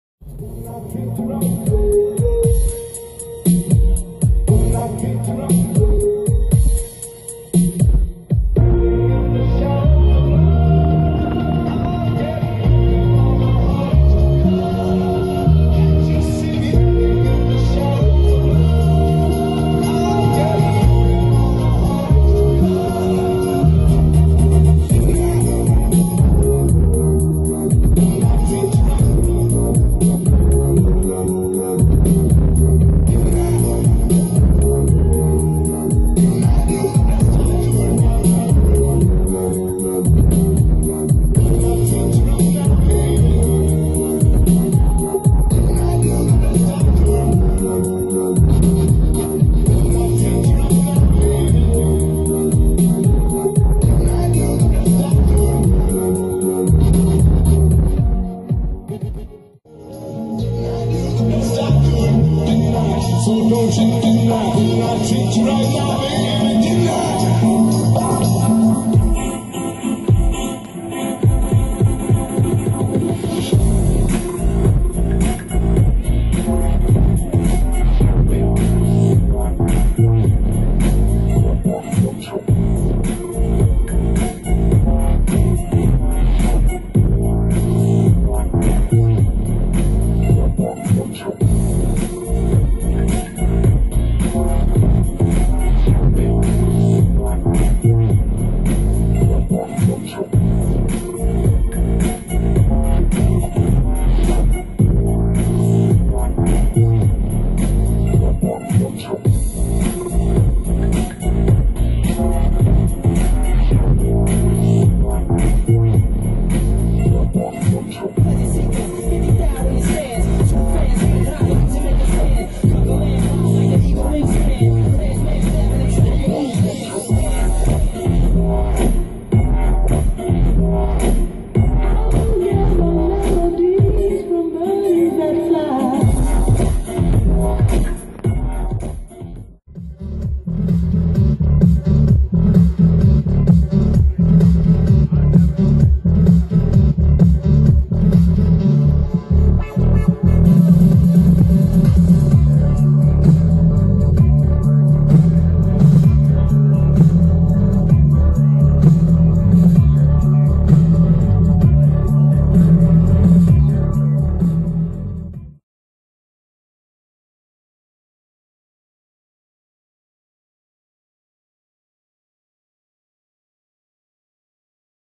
Woofstock Recap (8-30-15 Prince George, B.C.)
Some cell phone footage of my set.